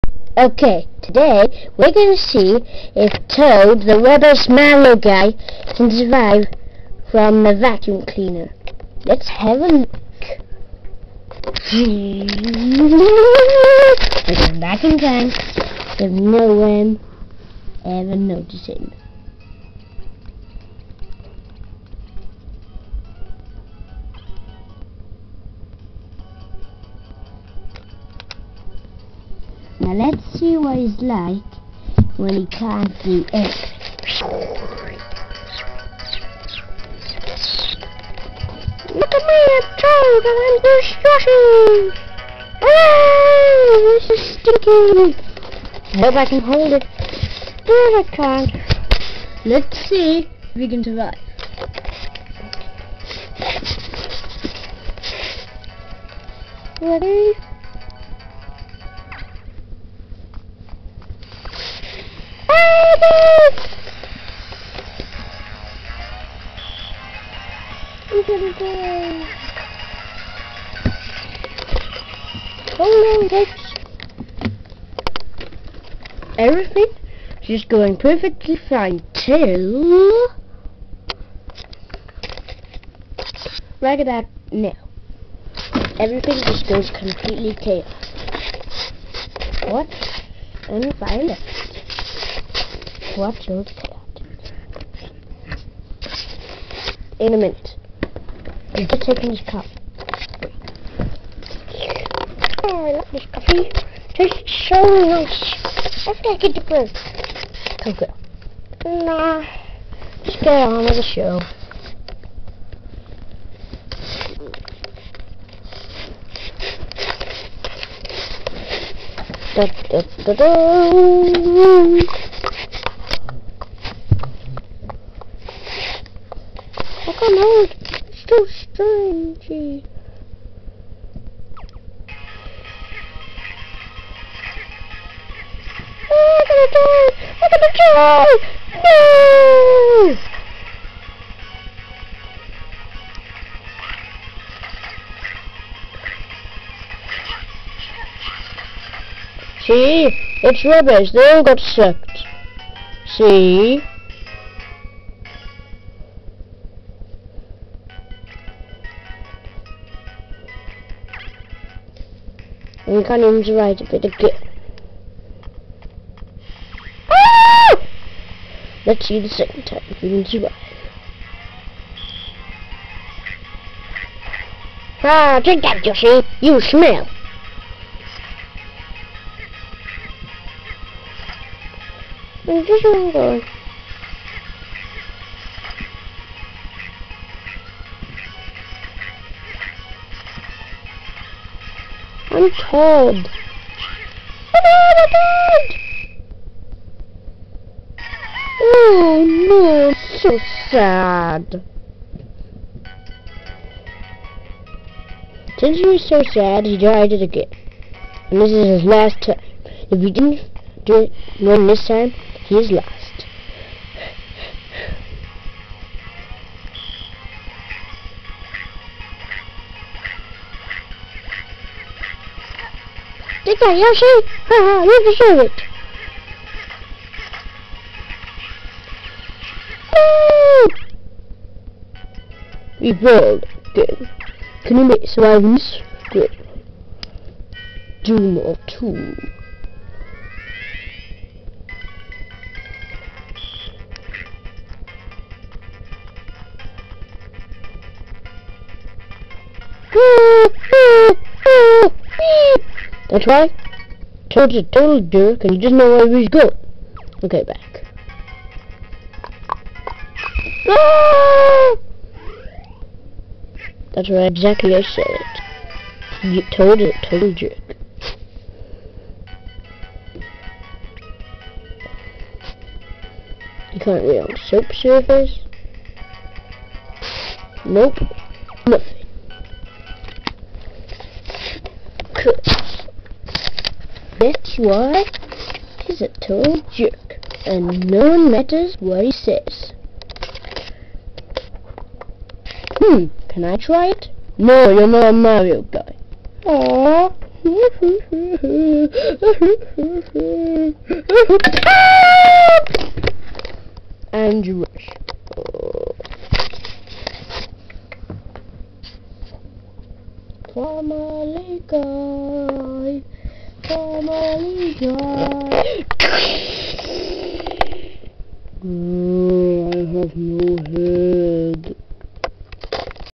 Ioan Protheroe presents ...."Toad the stinky guy" - an episode in film
the story of a stupid mushroom called Toad.